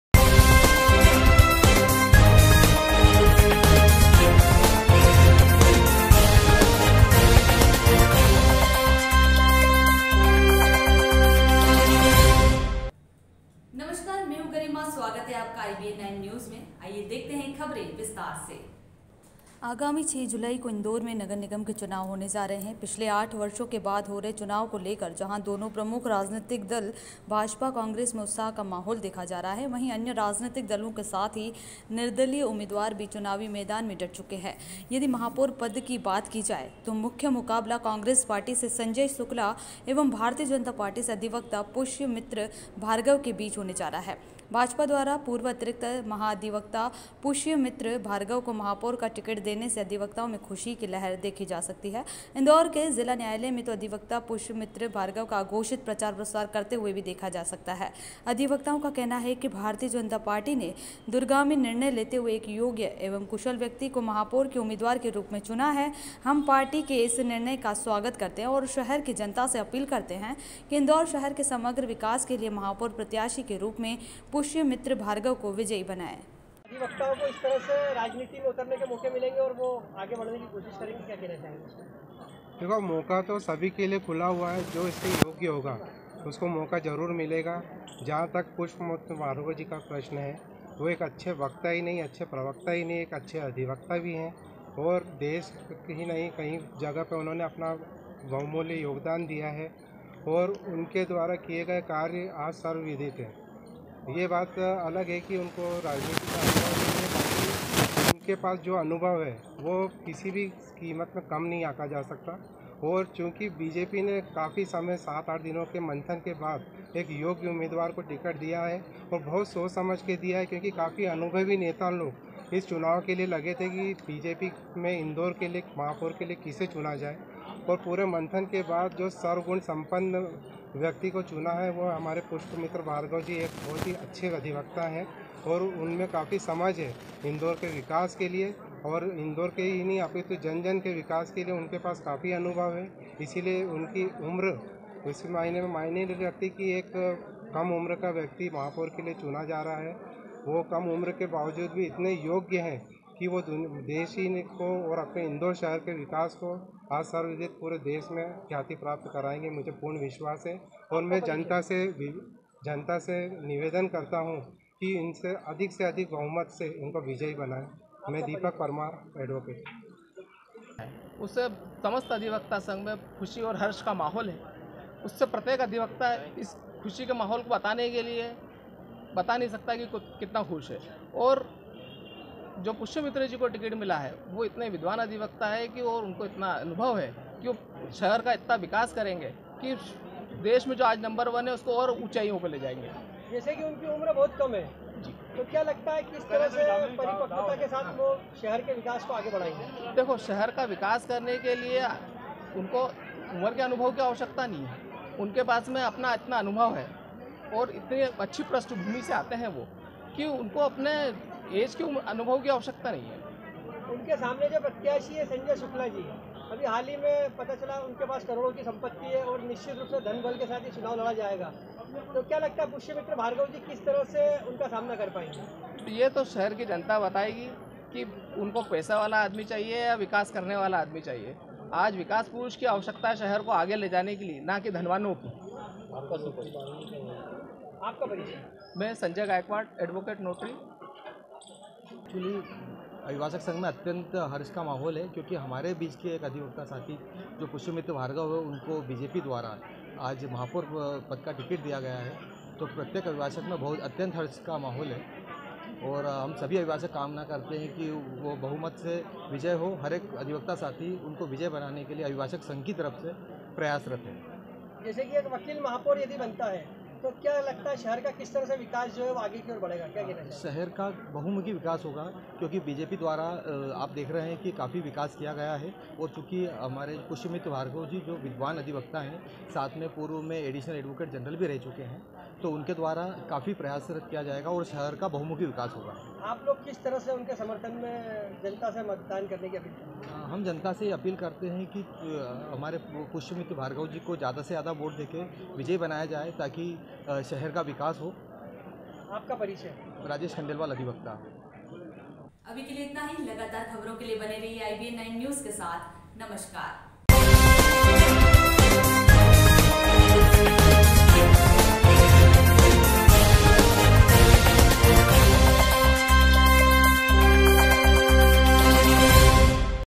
अधिवक्ता के महापौर का चुनाव लड़ते ही अभिभाषकों में उत्साह की लहर... | IBN9
अधिवक्ता के महापौर का चुनाव लड़ते ही अभिभाषकों में उत्साह की लहर...